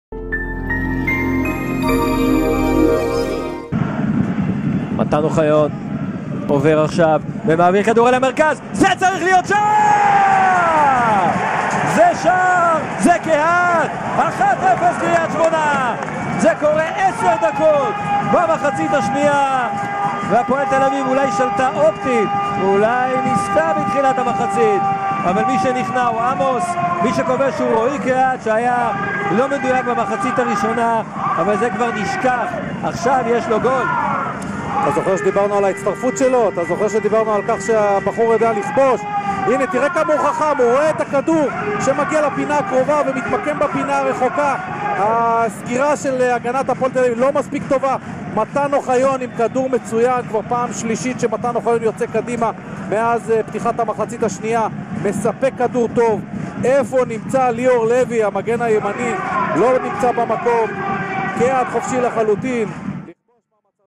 עירוני קרית שמונה - הפועל תל אביב 0:1 - רועי קהת עם שער בכורה! 19.4.14
דקה 55: ק"ש עלתה ל-0:1. בניגוד למהלך המשחק, החבורה של בכר ביתרון. מתן אוחיון העביר מאגף ימין כדור קשתי נהדר, עאבד פספס אבל קהת הגיע מצוין לכדור ושלח אותו לרשת של עמוס. שער נהדר לכישרון הצעיר של ק"ש.

Roy Kahat score the goal for Kiryat Shmona and his first goal in the league after a good assist from Matan Ohayon